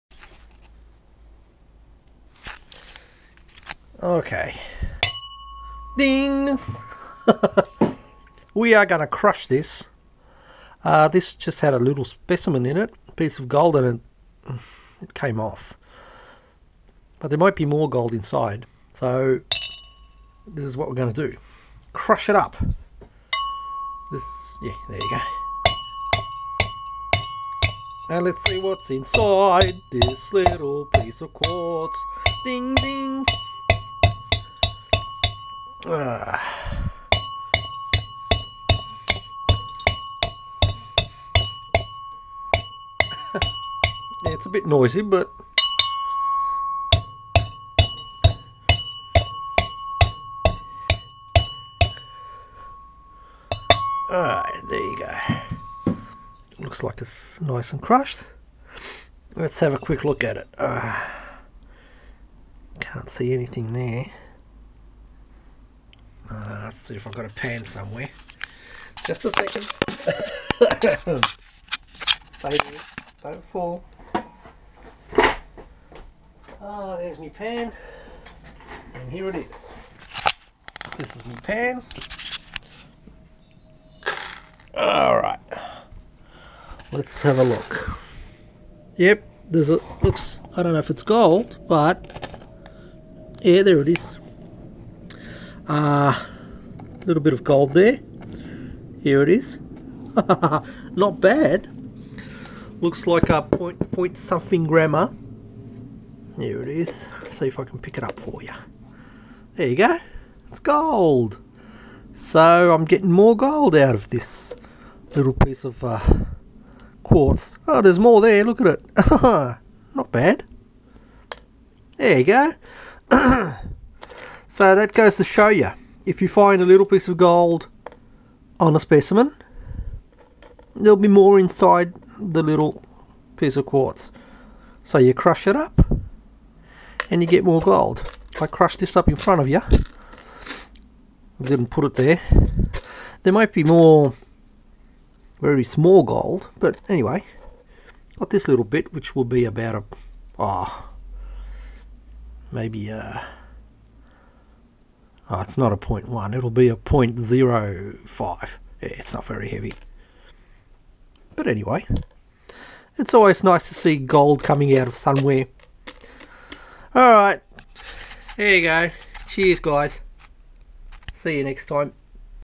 ...more crushing of Quartz and gold nugget found inside
More gold inside a quartz specimen

If you'd like to know where to grab some gold in  Creswick Forest, take a look at my Maps Book on Amazon .com: